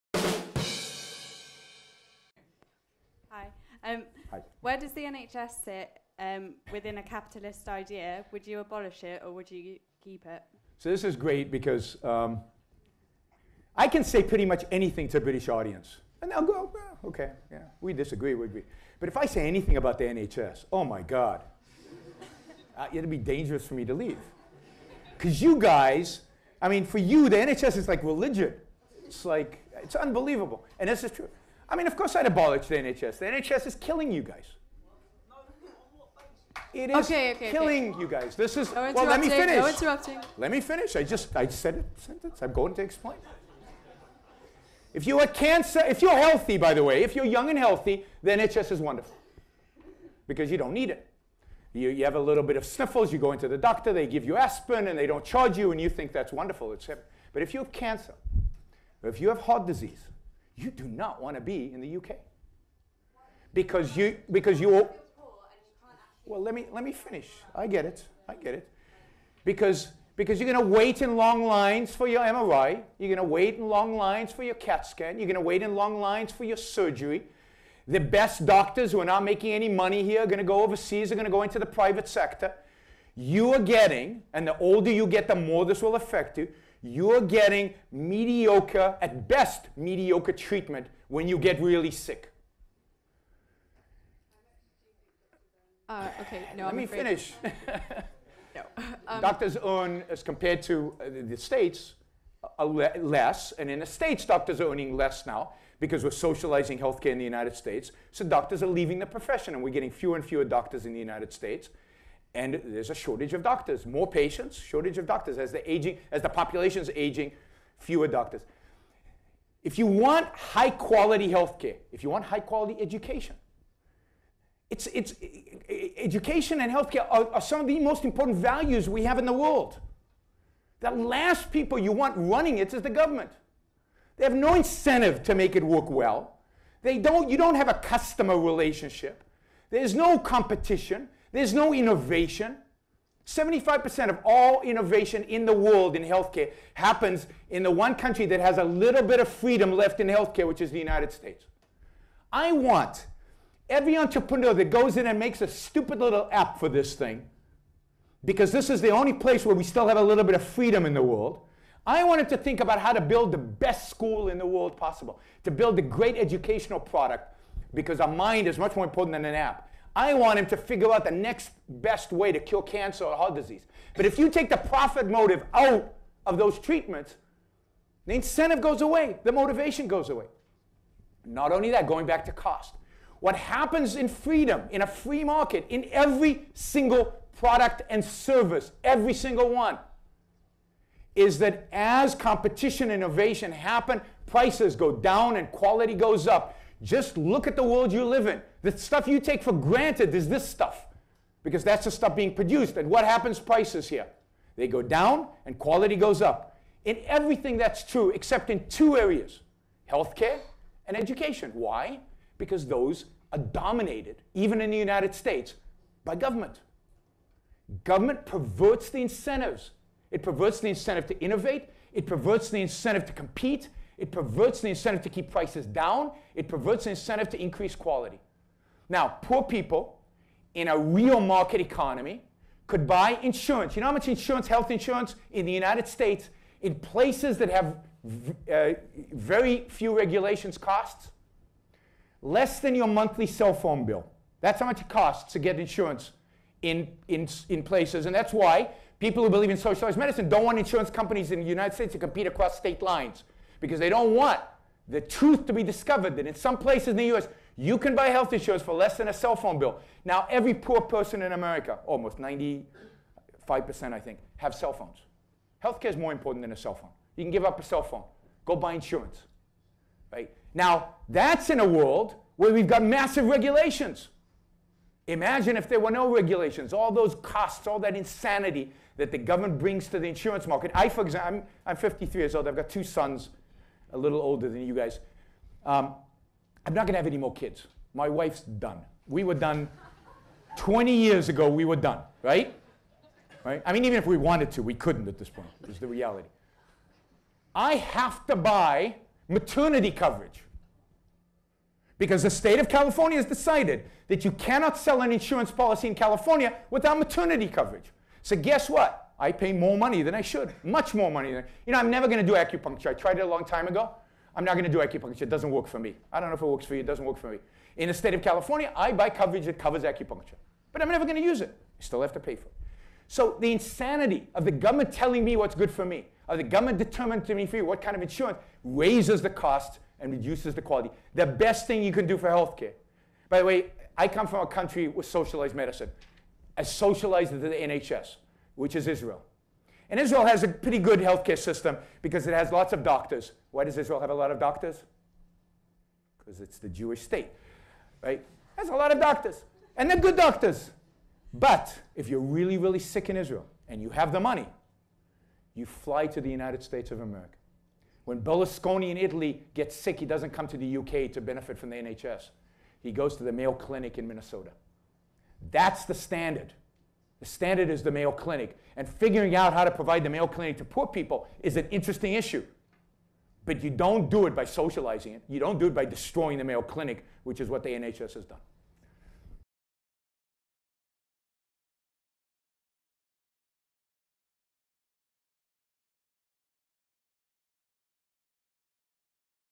Yaron Brook shocks a student: "England's National Health Service is killing you"
He also proves it!